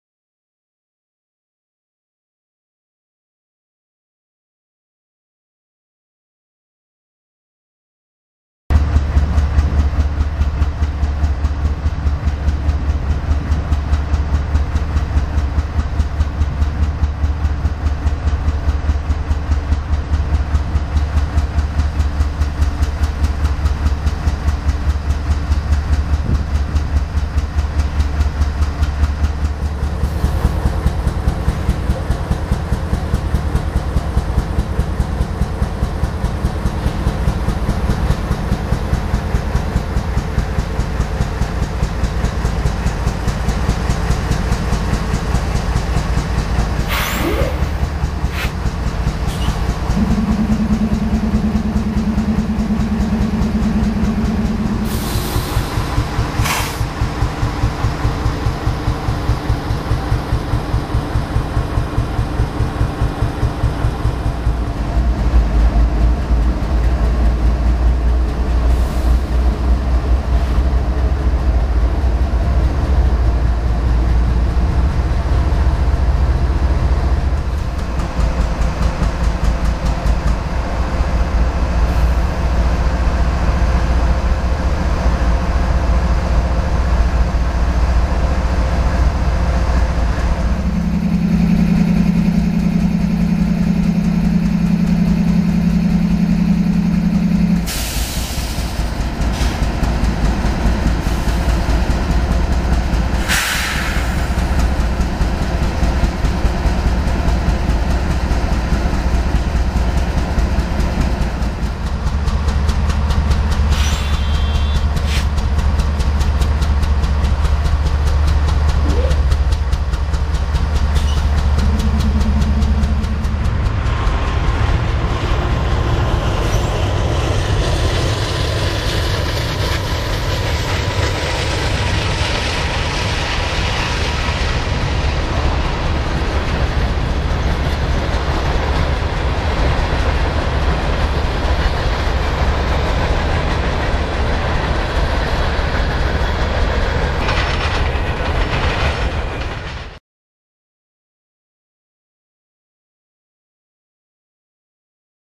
New Union Pacific GE Locomotives Idling at Erie, PA. Revised Footage. 2-14-09. Time 1:31pm.
This is Revised Video Footage of a Video that I have already posted on my youtube channel. I added a few more scenes plus the picture is clearer. In this video Brand New Union Pacific ES44AC Locomotives idle and chug away at Erie, Pennsylvania awaiting delivery to their new owner. Norfolk Southern Railroad will forward these New Locomotives west to the Union Pacific Railroad. Video recorded with permission including in cab footage and owned by MarkJL261 Productions. Enjoy!